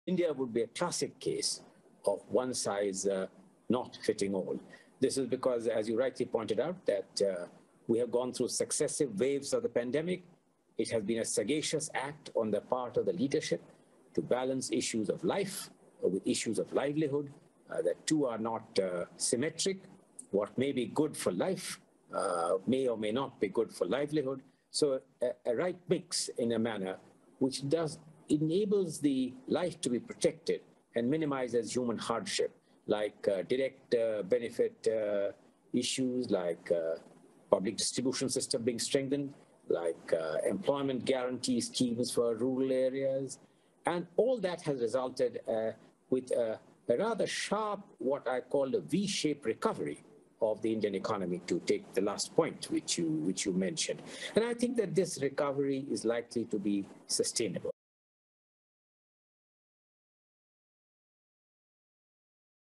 N.K. Singh | India is a Classic Case of One Size Not Fitting All
Speaking at the Sustainable Development Impact Summit 2021 during Shaping a Sustainable Recovery.